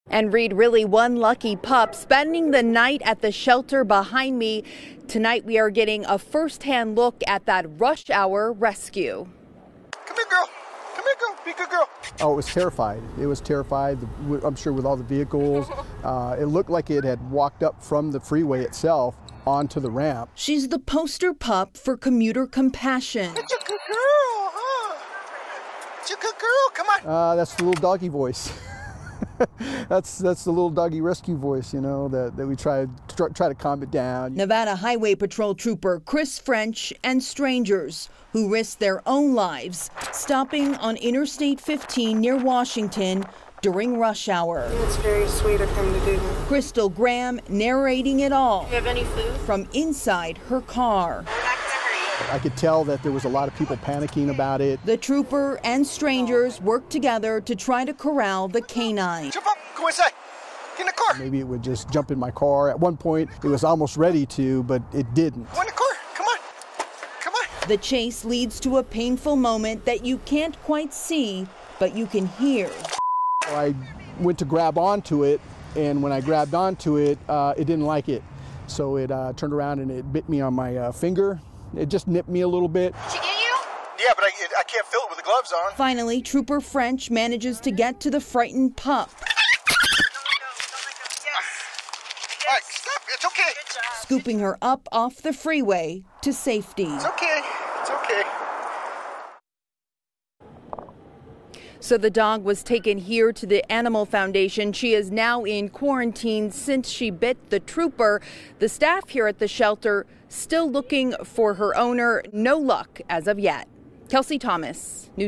Rush Hour Rescue: NHP Trooper saves dog running on I-15 freeway
A tiny dog whose rush hour rescue by Nevada Highway Patrol went viral on Twitter is now at The Animal Foundation.

Newly released body camera video of the rescue shows Trooper Chris French and rush hour drivers trying to save the wayward dog.

The chase happened just before 5 p.m. April 17 on Interstate 15 near the Washington exit.

In the video, Trooper French can be heard talking to the dog and other drivers throughout the entire ordeal.